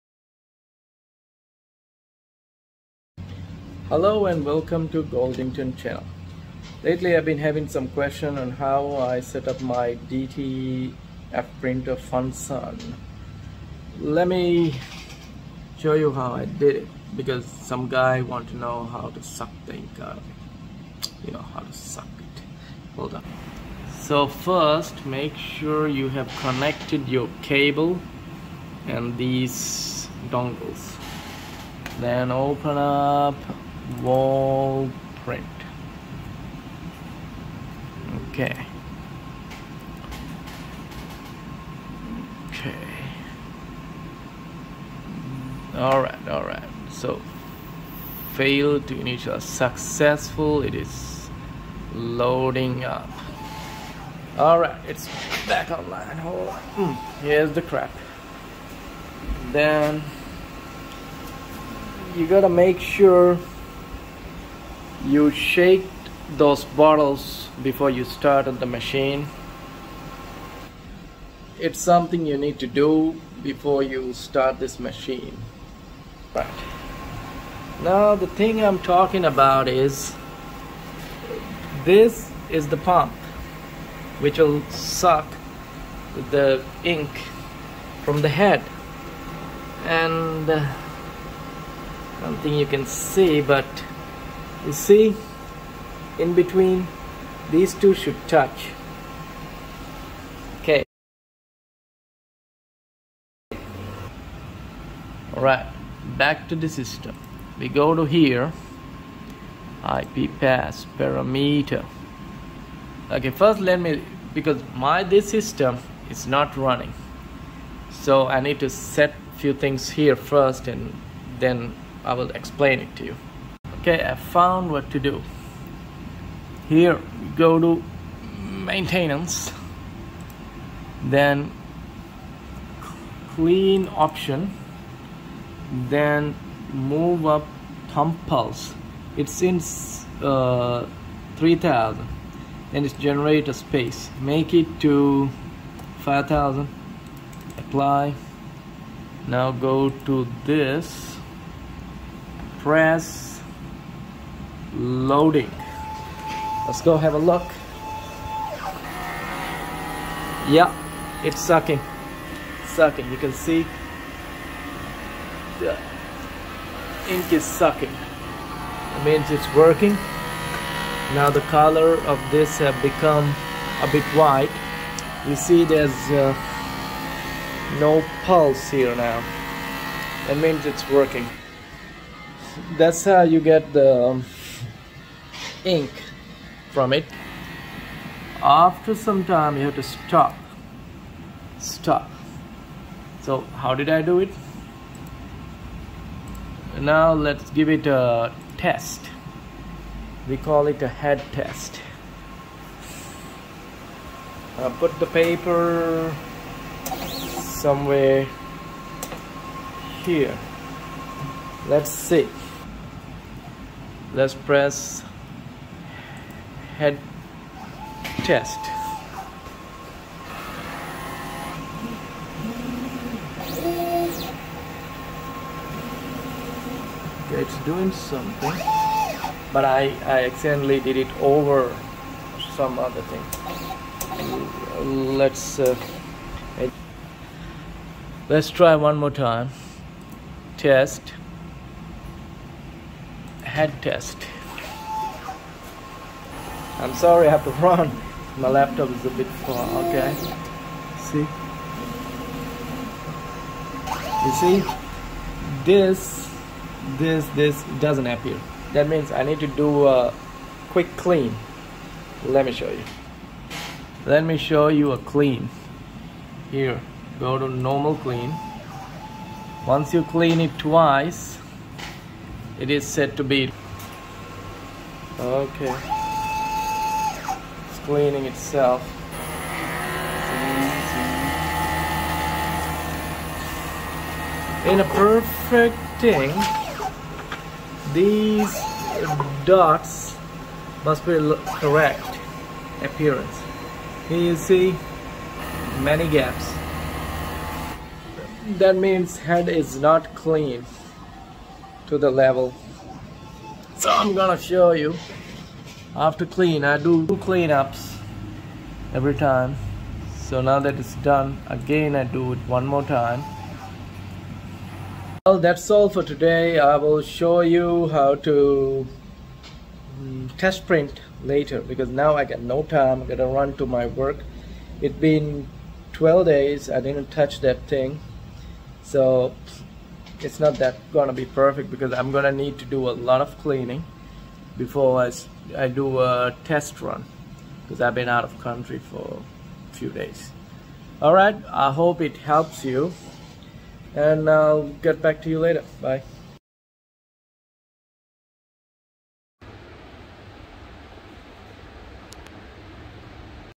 FUNSUN DTF printer how to solve ink problem
ive creared this vedio on a request to show how to suck into DTF printer head.